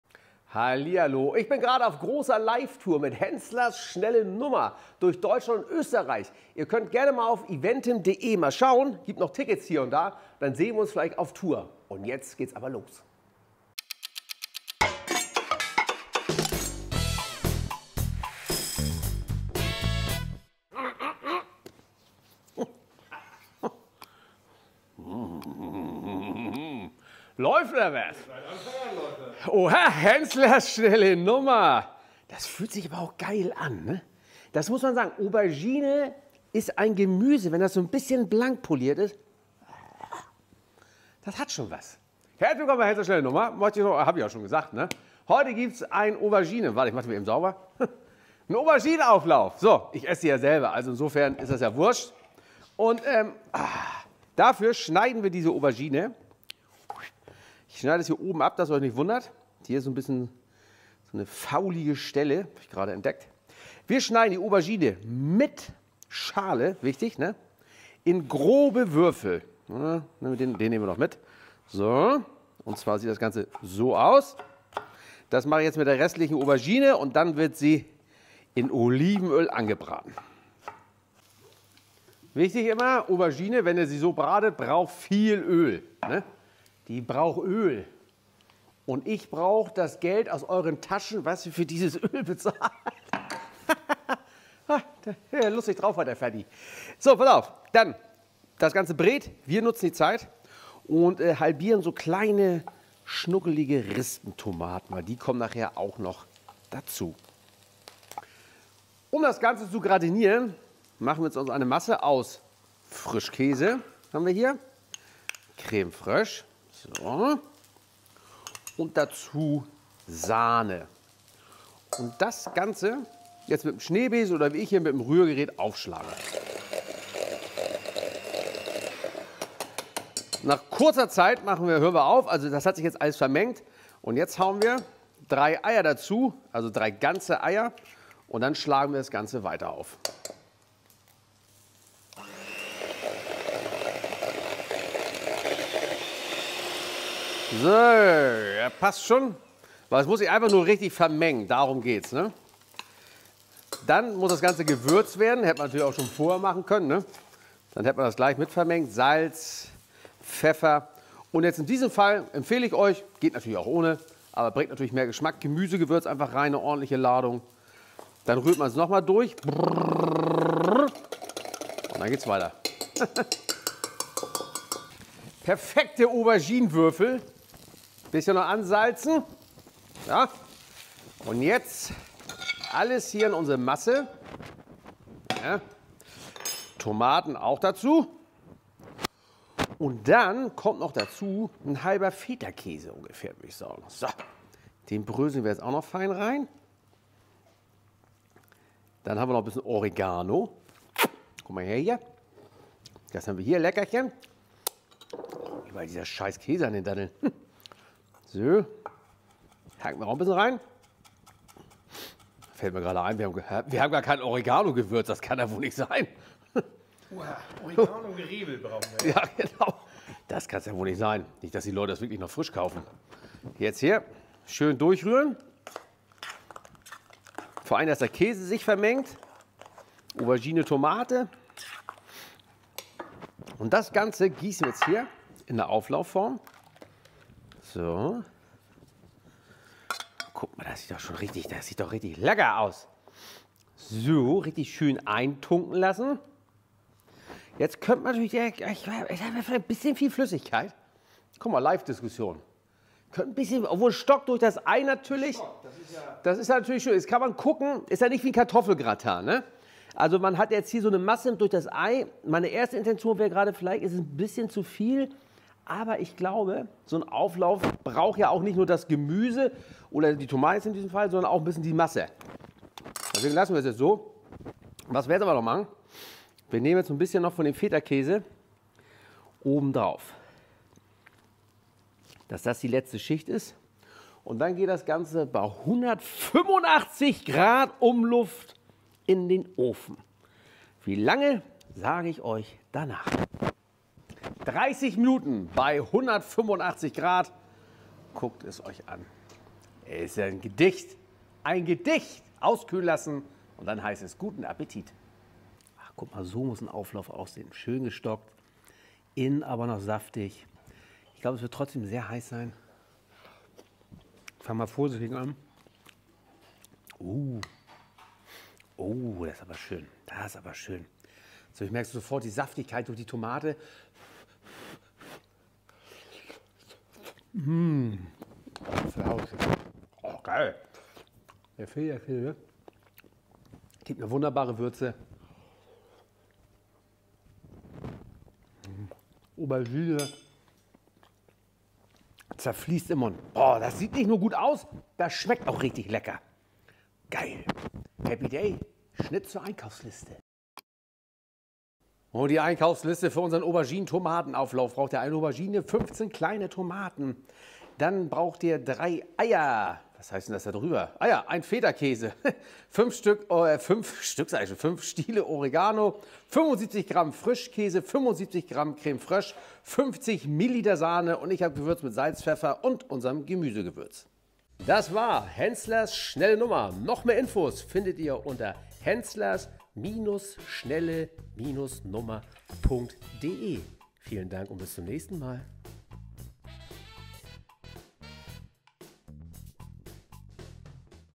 Schnelles Auberginen-Tomaten-Auflauf Rezept von Steffen Henssler
Das schnelle Auberginen-Tomaten-Auflauf Rezept vom Henssler - Schnell und einfach selbst kochen! Mehr schnelle Gerichte und Rezepte mit Video-Anleitungen findet Ihr

So geht‘s:

Backofen auf 185 °C Umluft vorheizen

Aubergine in grobe Würfel schneiden und mit dem Olivenöl in eine Pfanne geben. Auberginenwürfel im heißen Öl braten. In der Zwischenzeit die Tomaten halbieren

Frischkäse, Crème fraîche und Sahne in eine Schüssel geben und mit den Schneebesen des Handrührgeräts aufschlagen. Eier dazugeben und das Ganze weiter aufschlagen. Mit Gewürzsalz, Pfeffermix und Gemüsegewürz würzen und alles nochmals kurz durchrühren

Auberginenwürfel in der Pfanne mit Gewürzsalz würzen, kurz durchschwenken und anschließend zur Frischkäse-Sahne-Mischung in die Schüssel geben. Halbierte Tomaten mit in die Schüssel geben und halben Feta hineinbröckeln. Oregano fein herunterschneiden und ebenfalls mit in die Schüssel geben. Dann alles nochmals durchschwenken, in eine Auflaufform füllen, übrigen Feta darüberbröseln und das Ganze im heißen Ofen ca. 30 Minuten backen. Fertigen Auflauf aus dem Ofen nehmen und kurz abkühlen lassen

Einkaufsliste für 2 Personen:

1 Aubergine
4-6 EL HENSSLERS Olivenöl
15 kleine Tomaten
75 g Frischkäse
75 g Crème fraîche
50 ml Sahne
3 Eier
HENSSLERS Gewürzsalz
HENSSLERS Pfeffermix
HENSSLERS Gemüsegewürz
1 Stück (200 g) Fetakäse
5 Stiele Oregano